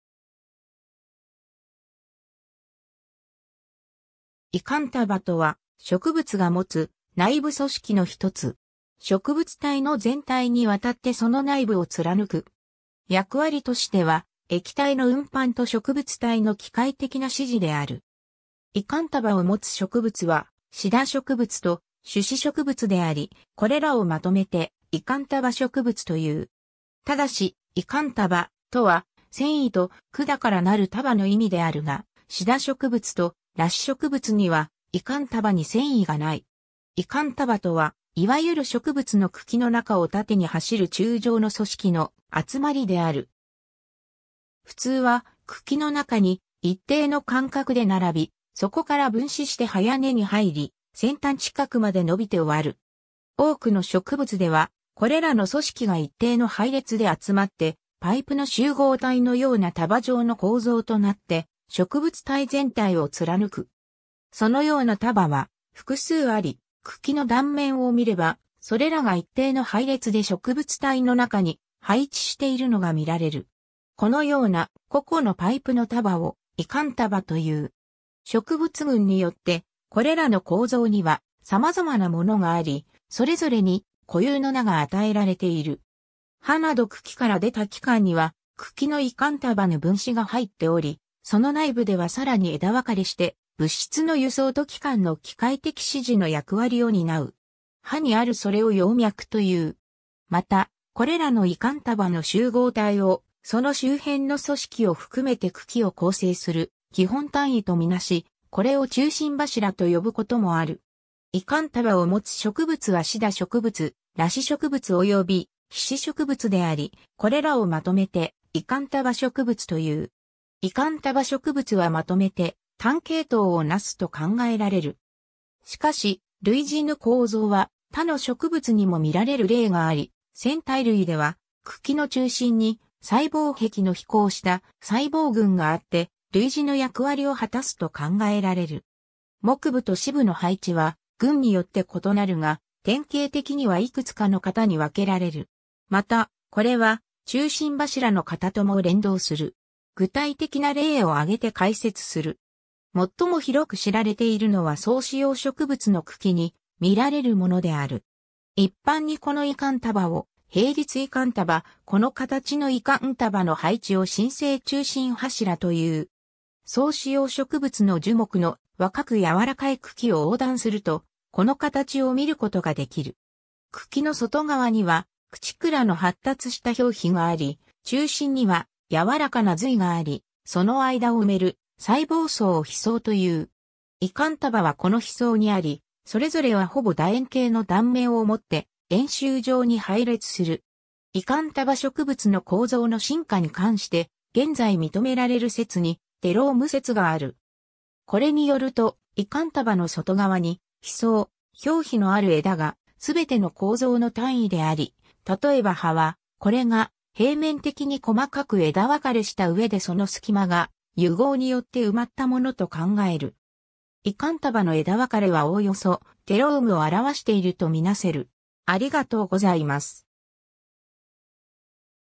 維管束
組織_(生物)
維管束（いかんそく、英語: vascular bundle）とは、植物が持つ内部組織の1つ。
植物体の全体に亘ってその内部を貫く。
役割としては液体（主に水や養分）の運搬と植物体の機械的な支持である。
維管束を持つ植物は、シダ植物と種子植物であり、これらをまとめて維管束植物という。
ただし、"維管束"とは繊維と管からなる束の意味であるがシダ植物と裸子植物には維管束に繊維が無い。
 維管束とは、いわゆる植物の茎の中を縦に走る柱状の組織の集まりである。
普通は茎の中に一定の間隔で並び、そこから分枝して葉や根に入り、先端近くまで伸びて終わる。
 多くの植物では、これらの組織が一定の配列で集まってパイプの集合体のような束状の構造となって植物体全体を貫く。
そのような束は、複数あり、茎の断面を見れば、それらが一定の配列で植物体の中に配置しているのが見られる。
このような、個々のパイプの束を維管束と言う。
植物群によってこれらの構造には様々なものがあり、それぞれに固有の名が与えられている。
 葉など茎から出た器官には茎の維管束の分枝が入っており、その内部ではさらに枝分かれして物質の輸送と器官の機械的支持の役割を担う。
葉にあるそれを葉脈という。
 また、これらの維管束の集合体を、その周辺の組織を含めて茎を構成する基本単位と見なし、これを中心柱（ちゅうしんちゅう）と呼ぶこともある。
 維管束を持つ植物はシダ植物（広義）、裸子植物及び被子植物であり、これらをまとめて維管束植物という。
維管束植物はまとめて単系統をなすと考えられる。
しかし、類似の構造は他の植物にも見られる例があり、蘚苔類では茎の中心に細胞壁の肥厚した細胞群があって、類似の役割を果たすと考えられる。
 木部と師部の配置は、群によって異なるが、典型的にはいくつかの型に分けられる。
また、これは中心柱の型とも連動する。
 具体的な例をあげて解説する。
もっともひろく知られているのは双子葉植物の茎に見られるものである。
一般にこの維管束を並立維管束、この形の維管束の配置を真正中心柱という。
 双子葉植物の樹木の、若く柔らかい茎を横断すると、この形を見る事が出来る。
茎の外側にはクチクラの発達した表皮があり、中心には柔らかな髄があり、その間を埋める細胞層を皮層と言う。
維管束はこの皮層にあり、それぞれはほぼ楕円形の断面を持って、円周状に配列する。
 維管束植物の構造の進化に関して、現在認められる説にテローム説がある。
これによると、維管束の外側に皮層、表皮のある枝がすべての構造の単位であり、例えば葉はこれが平面的に細かく枝分かれした上でその隙間が癒合によって埋まったものと考える。
維管束の枝分かれはおおよそテロームをあらわしているとみなせる。